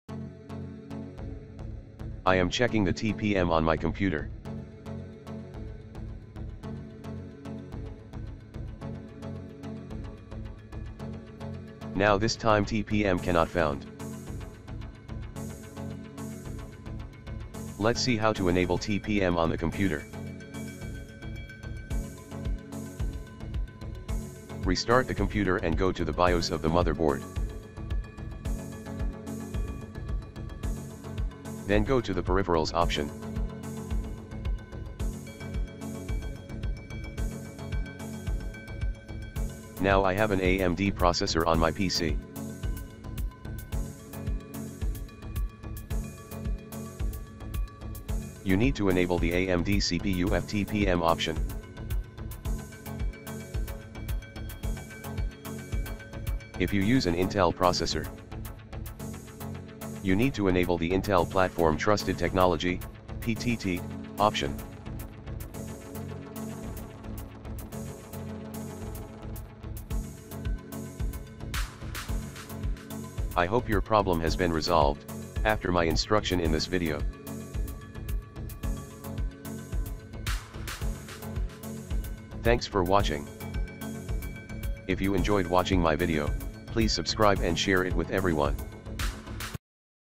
How to enable tpm in bios gigabyte (Intel and AMD)
I am checking the TPM on my computer. Now this time TPM cannot found. Let's see how to enable TPM on the computer. Restart the computer and go to the BIOS of the motherboard.
Then go to the Peripherals option.
Now I have an AMD processor on my PC. You need to enable the AMD CPU fTPM option.
If you use an Intel processor. You need to enable the Intel Platform Trusted Technology (PTT) option.